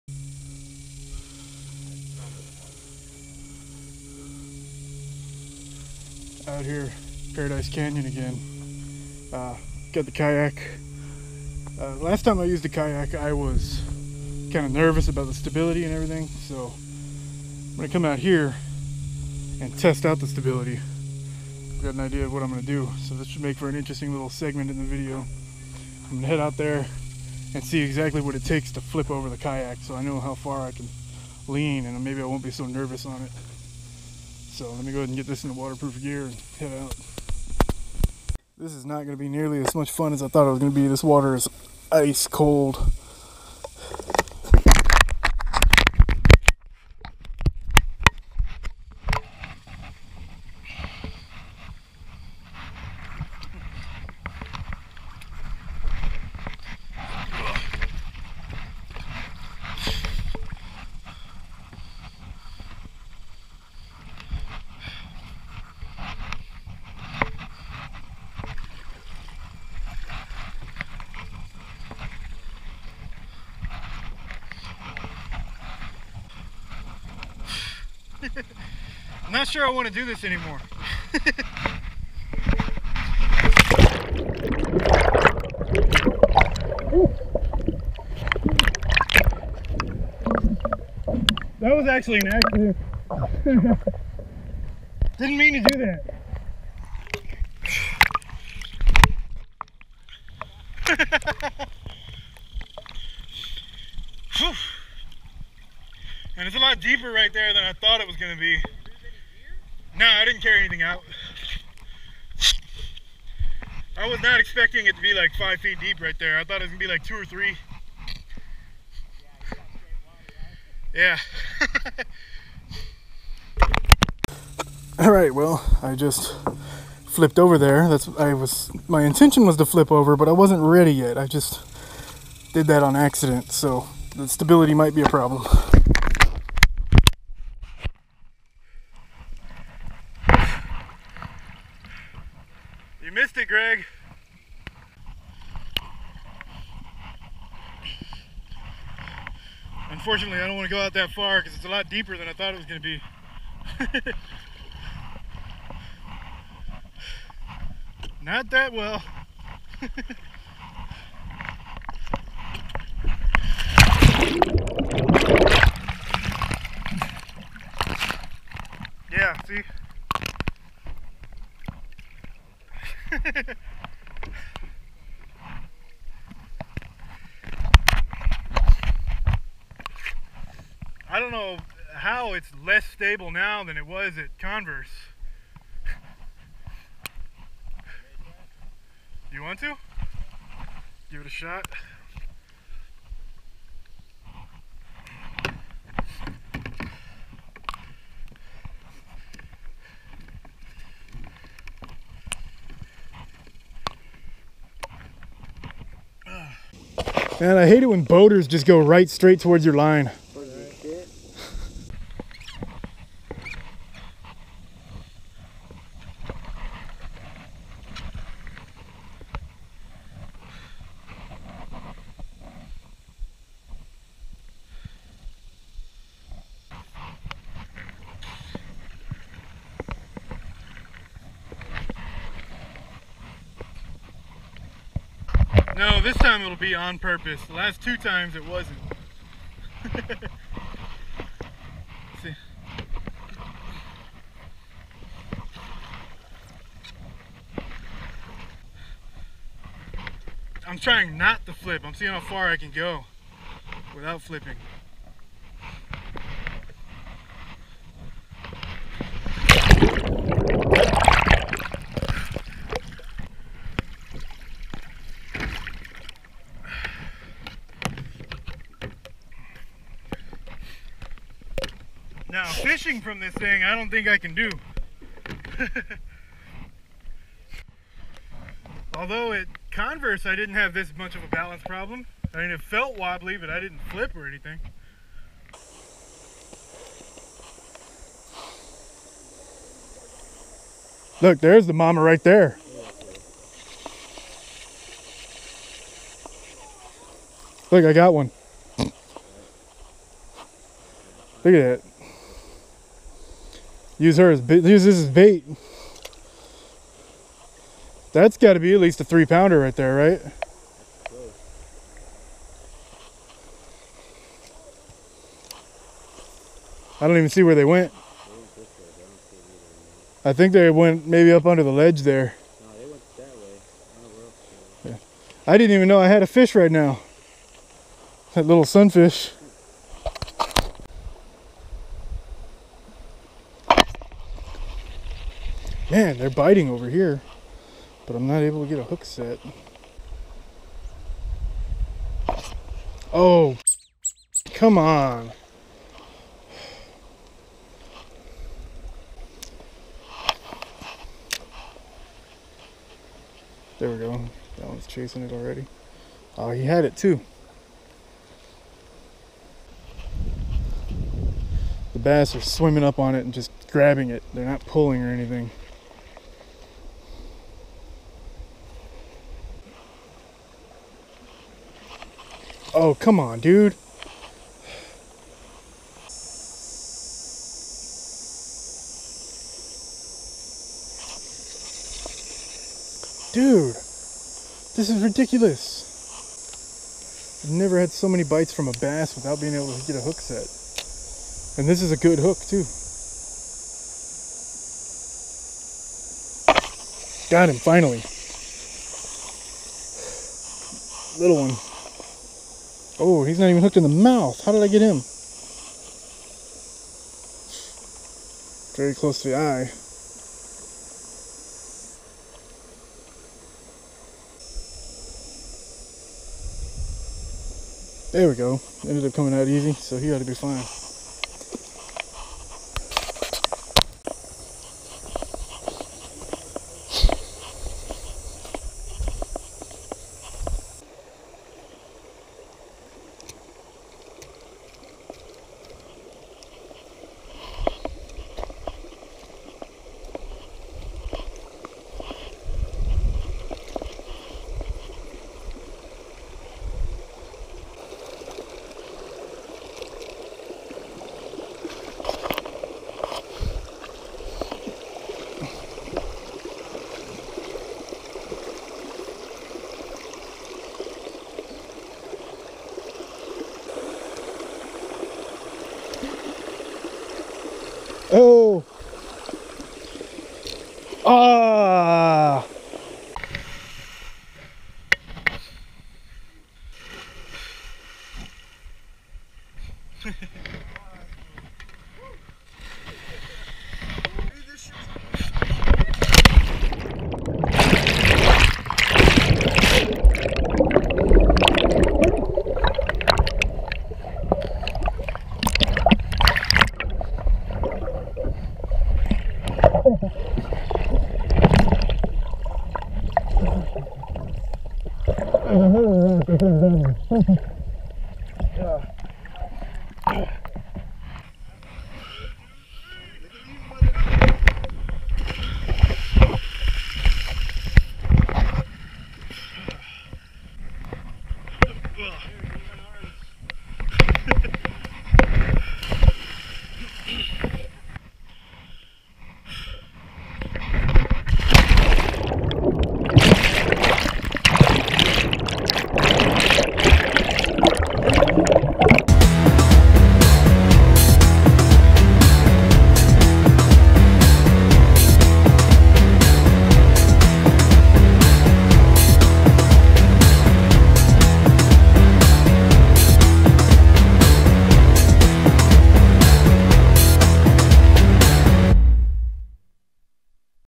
What Does It Take to Flip a Kayak?
The answer is: Not much at all. I've already got some outriggers on the way. It seems more stable when fully loaded with gear than completely empty.

Want to help my channel get much better? Or have influence over an upcoming video?